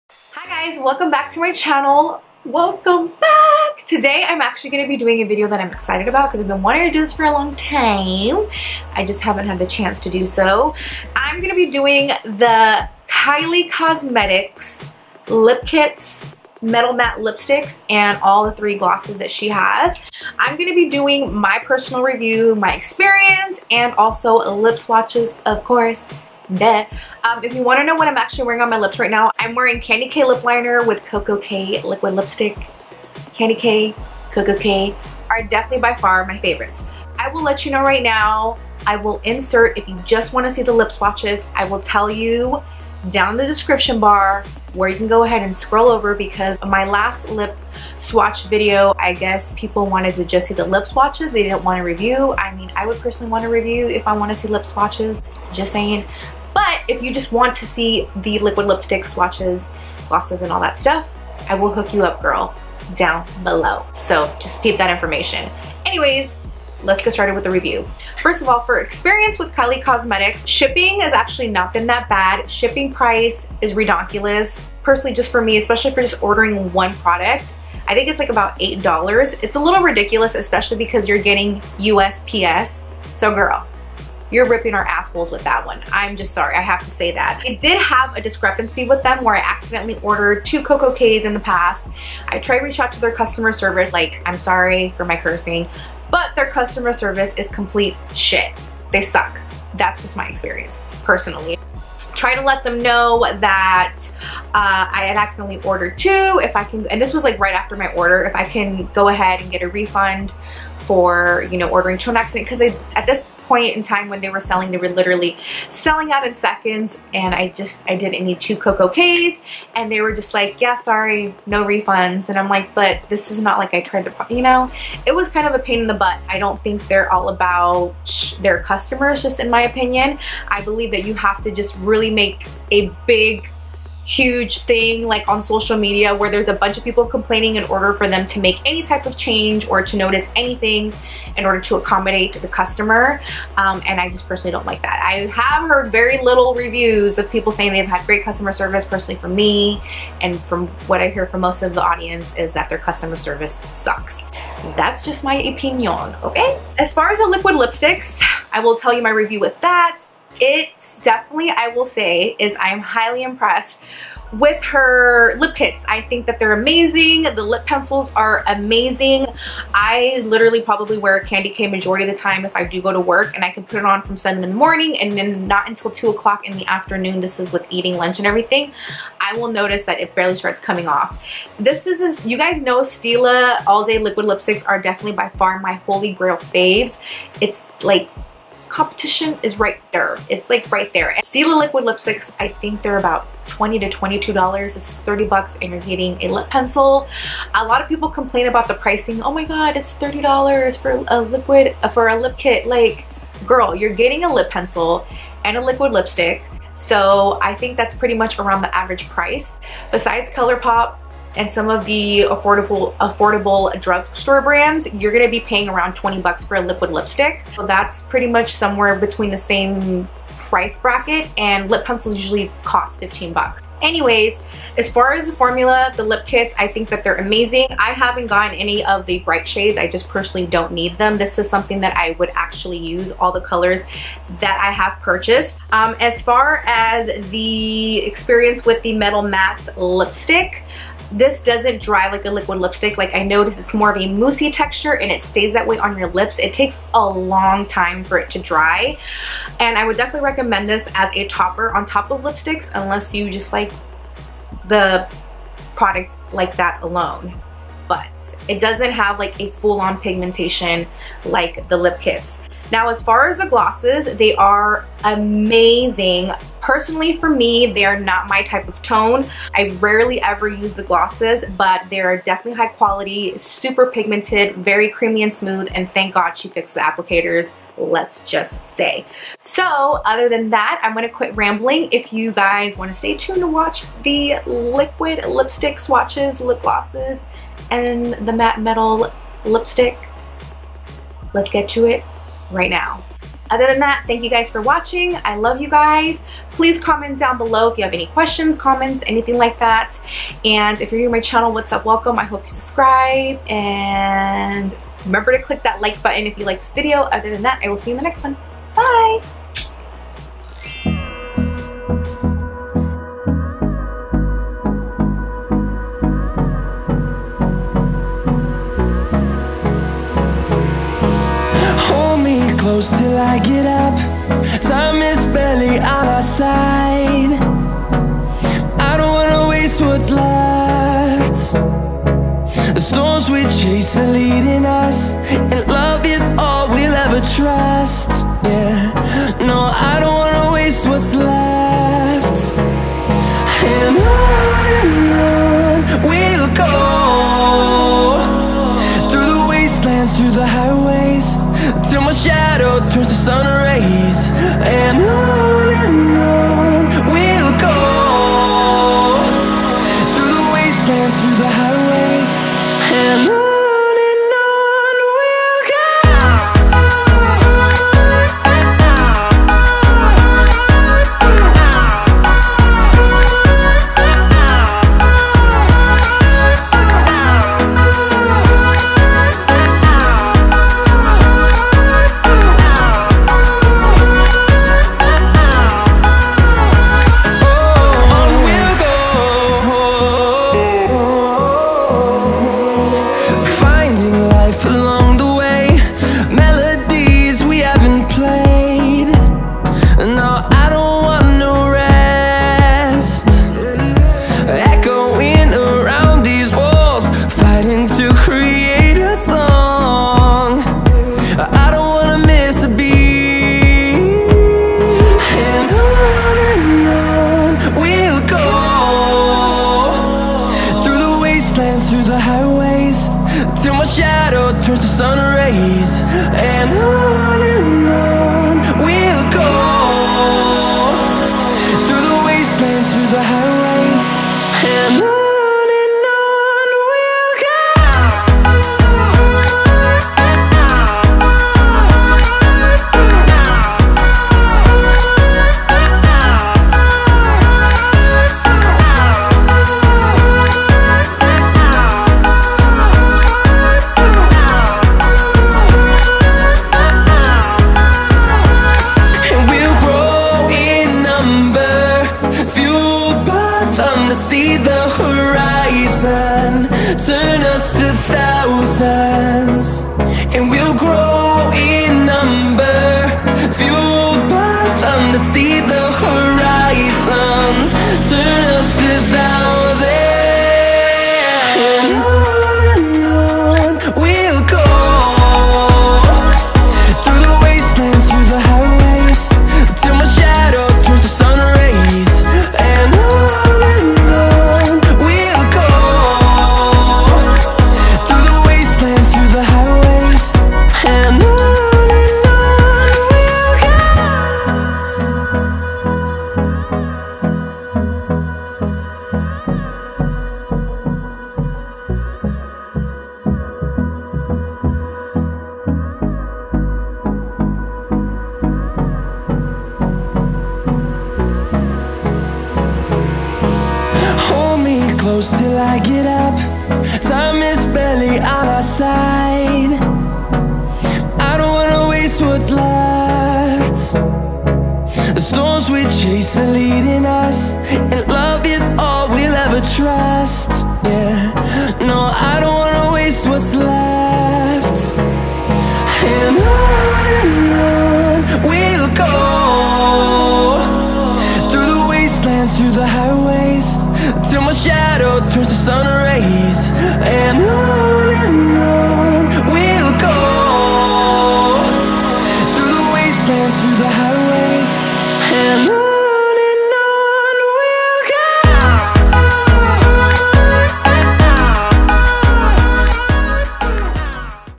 My Experience with Kylie Cosmetics: Review & Swatches
♡ Save some dinero on Sigma Brushes!!! ♡

If you want to get cash back for shopping try Ebates **OBSESSED**

♡ Products mentioned

LIP KITS

1.) Candy K
2.) Dolce K
3.) KoKo K
4.) Posie K

Metal Matte Lipstick

5.) Heir

Glosses

6.) So Cute
7.) Literally
8.) Like

♡ LOVE YOU SUBBIES! ♡